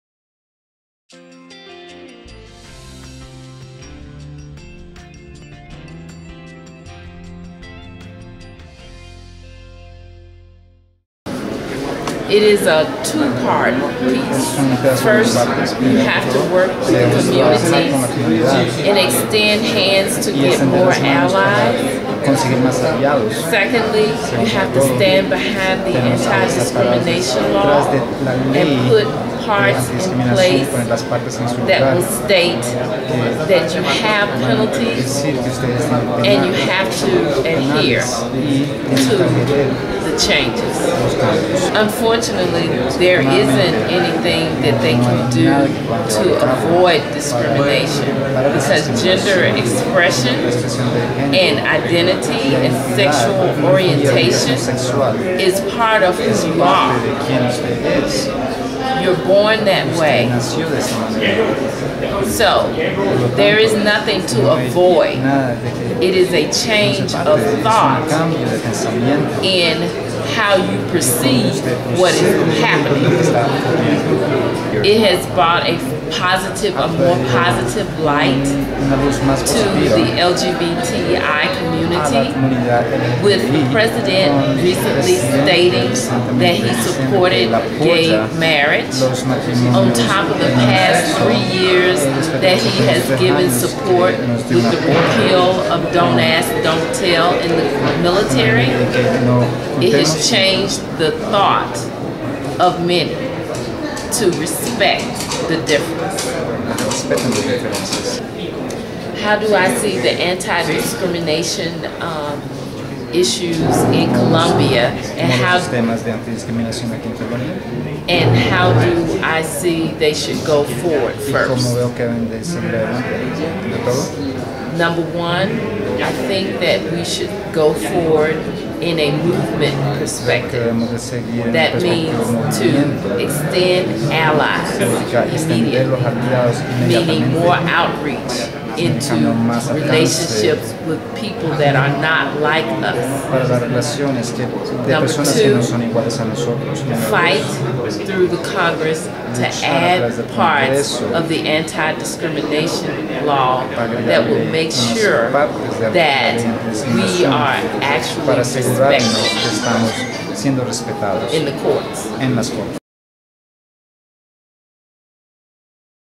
Racismo y discriminación en Colombia
Donna Payne, directora de Diversidad de Human Rigths Campaign, comparte su experiencia como activista antidiscriminación en Estados Unidos y ofrece luces sobre los pasos que deben dar los movimientos sociales en Colombia.